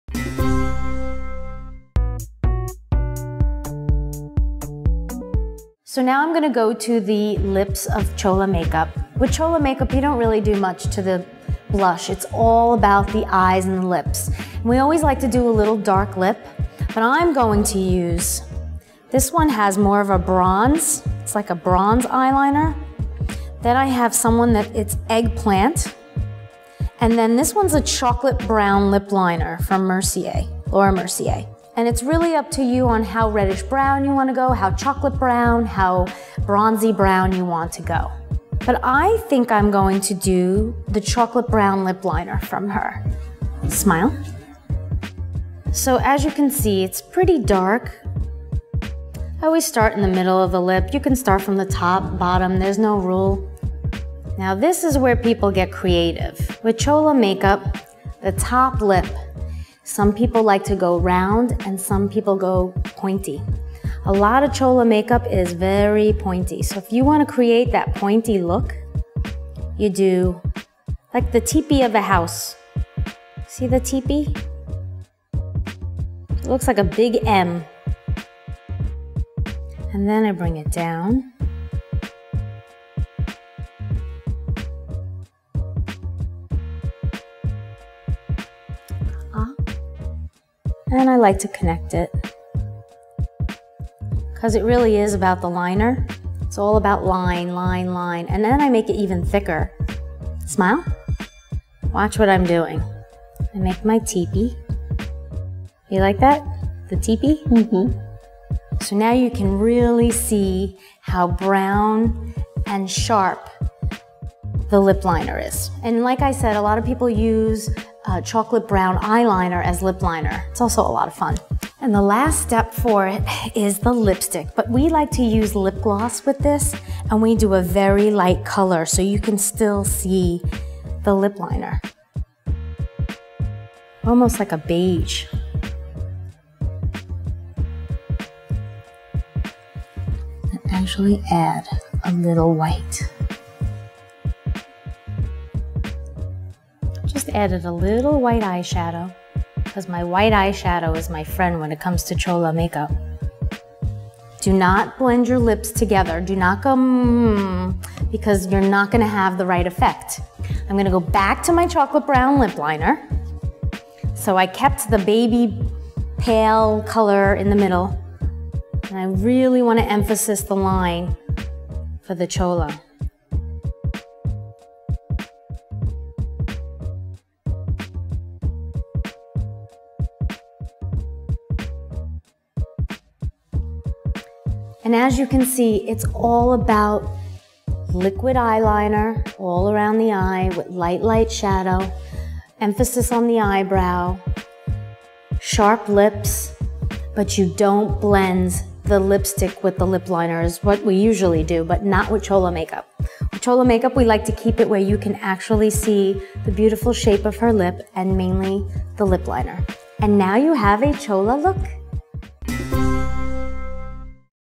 How to Do Chola Lips | Makeup Lessons
"So now I going to the lips of Chola makeup, with Chola makeup you don't really do much to the blush, it's all about the eyes and the lips and we always like to do a little dark lip. I'm going to use, this one has more of a bronze, it's like a bronze eyeliner. Then I has some that's eggplant and then this one's a chocolate brown lip liner from Mercier and it's really up to you on how reddish-brown you want to go, how chocolate brown, how bronzy brown you want to go.

But I think I'm going to do the chocolate brown lip liner from her. Smile. So as you can see it's pretty dark. I always start in the middle of the lip, you can start from the top, bottom, there's no rule. Now this is where people get creative, with Chola makeup the top lip some people like to go round and some people go pointy.

A lot of Chola makeup is very pointy. So if you want to create that pointy look you do like the tepee of a house, you see the tepee? It looks like a big M and then I bring it down and I like to connect it because it really is about the liner. It's all about line, line, line and then I make it even thicker. Smile. Watch what I'm doing, I make my tepee, you like that? The tepee? Uh huh.

So now you can really see how brown and sharp the lip liner is and like I said, a lot of people use a chocolate brown eye liner as lip liner. It's also a lot of fun. And the last step for it is the lipstick, but we like to use lip gloss with this and we do a very light color so you can still see the lip liner, almost like a beige. Actually add a little white, just added a little white eye shadow because my white eye shadow is my friend when it comes to Chola makeup.

Do not blend your lips together, do not go mmmm because you're not going to have the right effect. I'm going to go back to my chocolate brown lip liner, so I kept the baby pale color in the middle and I really want to emphasis the line for the Chola. And as you can see it's all about liquid eye liner all around the eye with light, light shadow, emphasis on the eyebrow, sharp lips but you don't blend the lipstick with the lip liner, which is what we usually do, but not with Chola makeup. Chola makeup we like to keep it where you can actually see the beautiful shape of her lip and mainly the lip liner and now you have a Chola look.